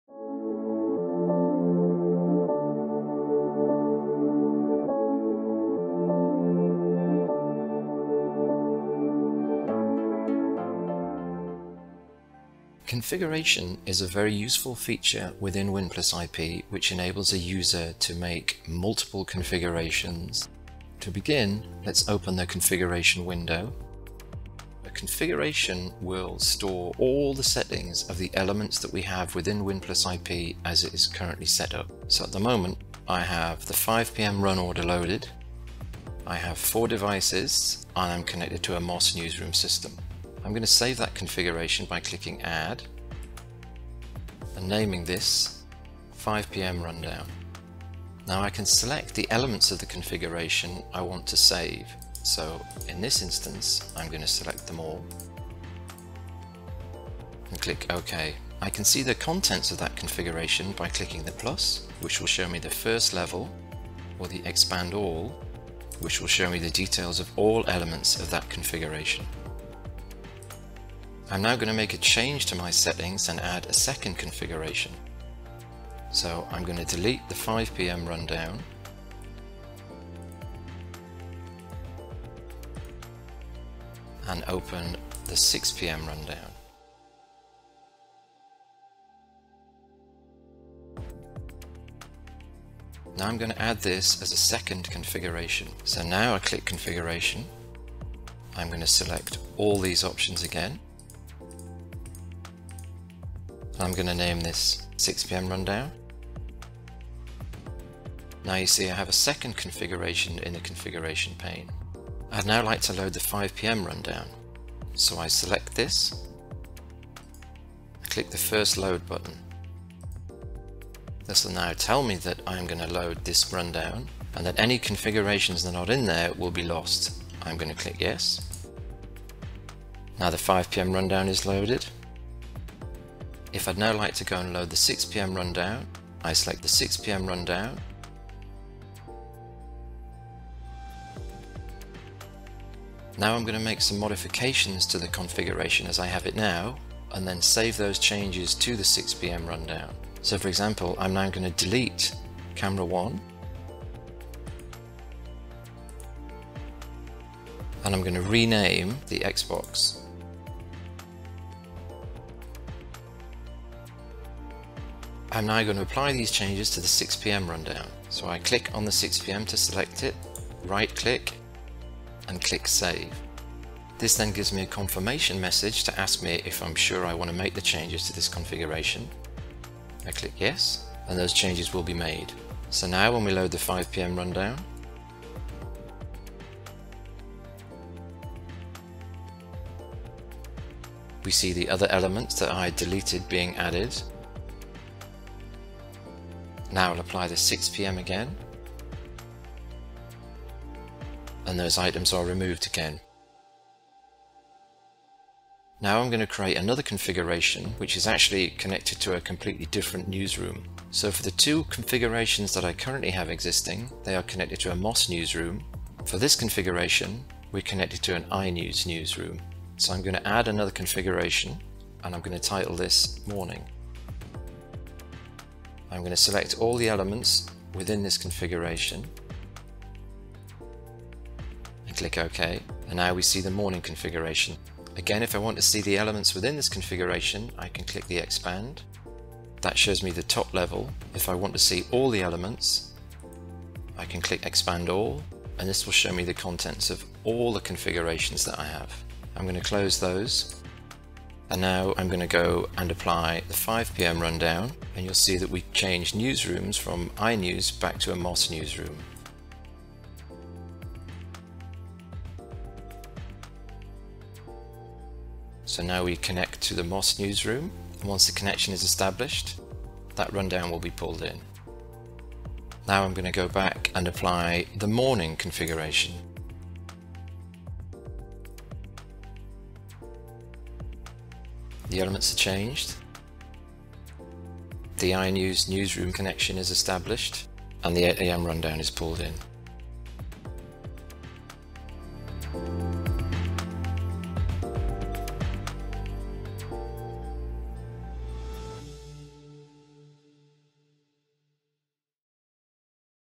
WinPlus-IP How To: Use Configuration
A short tutorial showing how to create and modify a library of configurations in WinPlus-IP. These can be quickly applied to switch between setups.